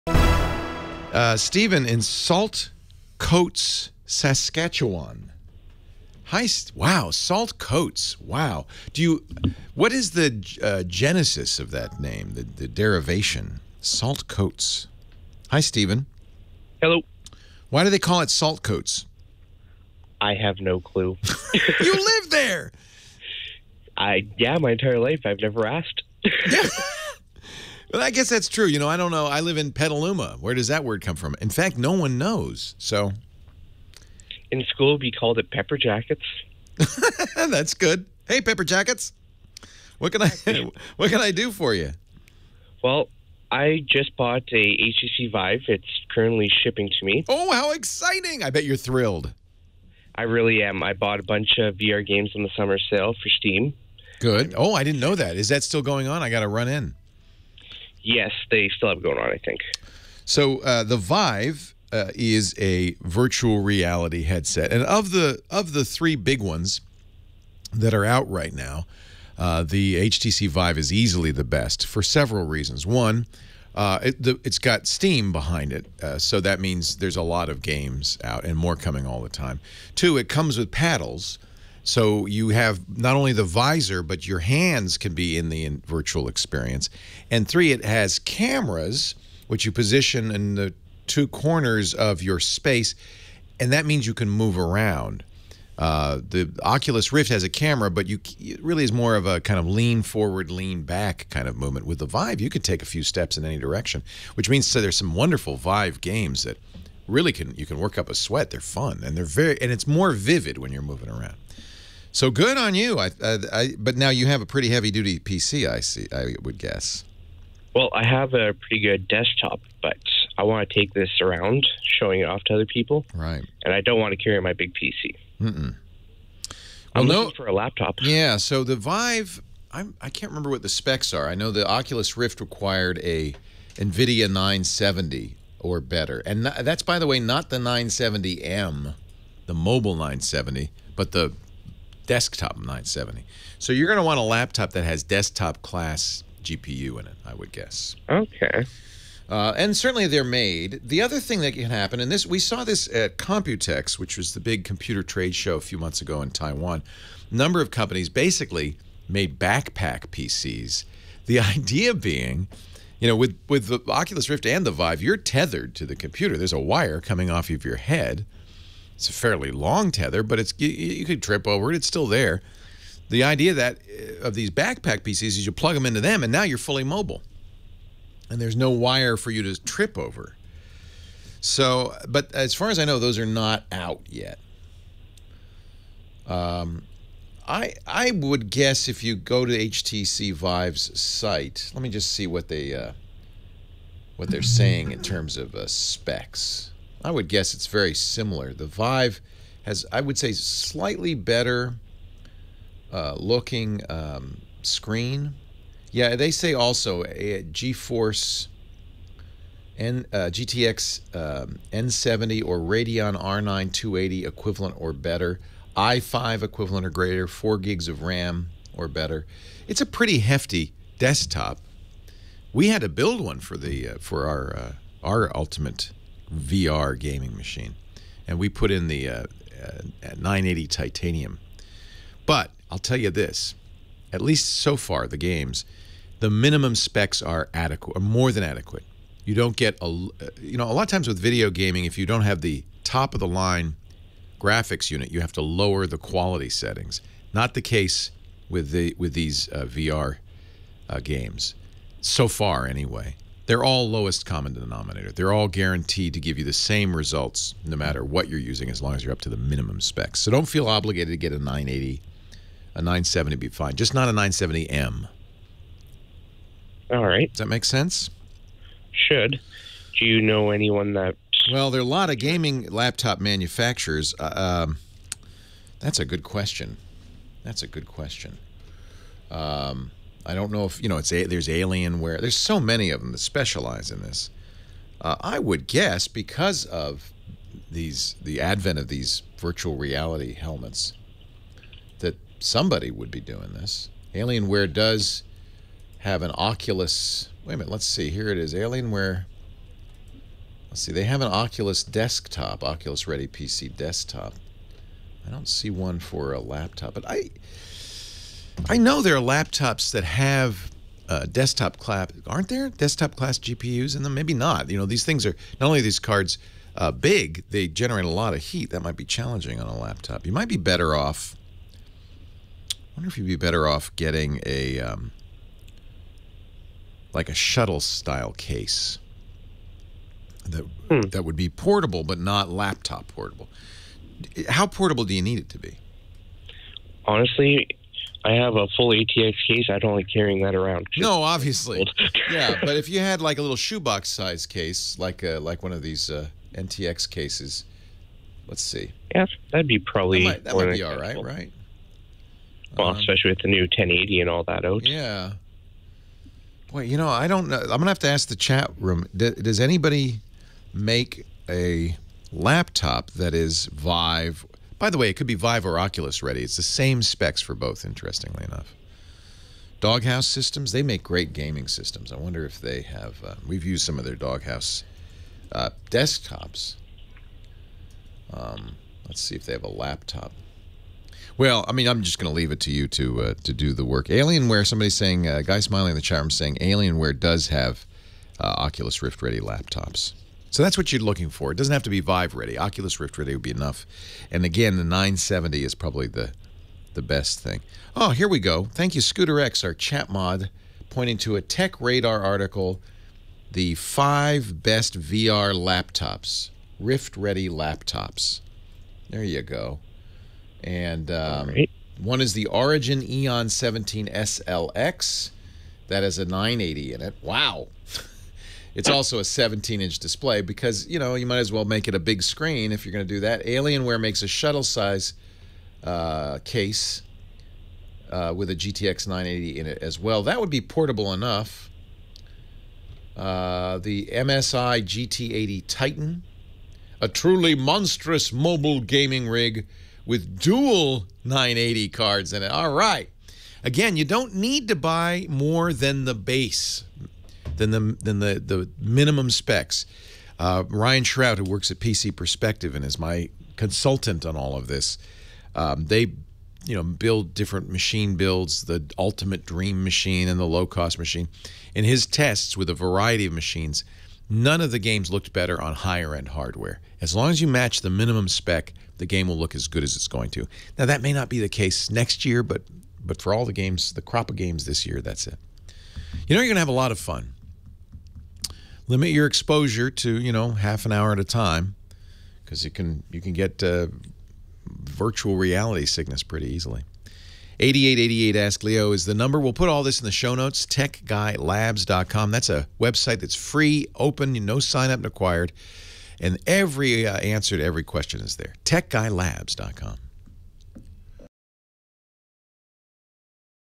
Portable Virtual Reality Gaming PCs
Leo Laporte talks about the minimum requirements for virtual reality games and how to get a more mobile rig that can handle it.